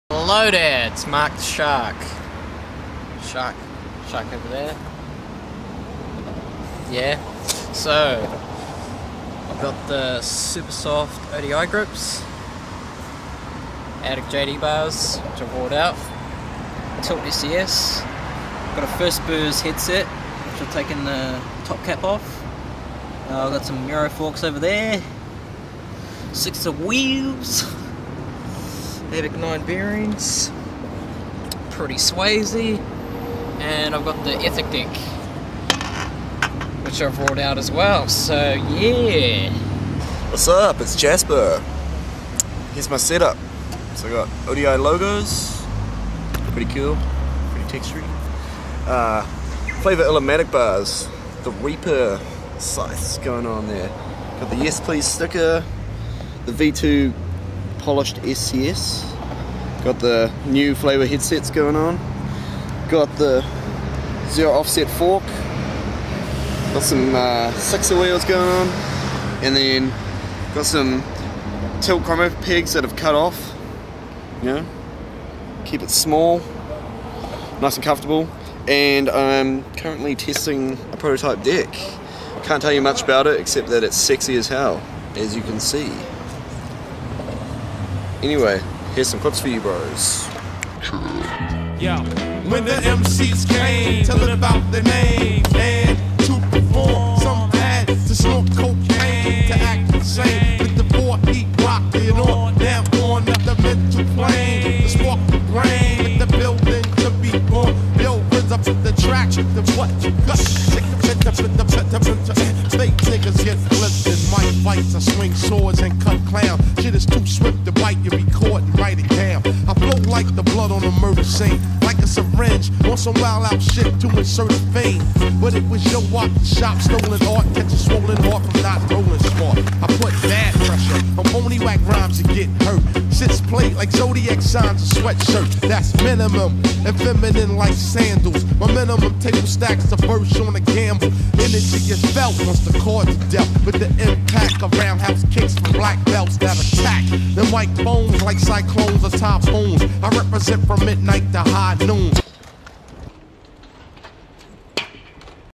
Mark Abbott & Jasper Russell-Dennis | Scooter Check
Watch the OG's from NZ, as they go through their current setups. Be sure to keep an eye out for more footage from these lads in the near future...